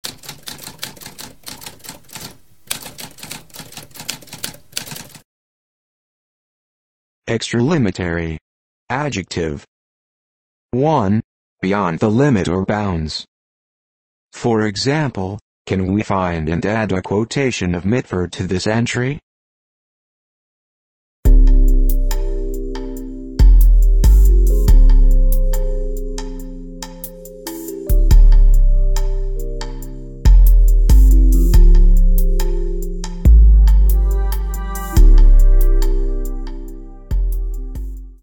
What does extralimitary mean?
A spoken definition of extralimitary.

Intro Sound:
Typewriter - Tamskp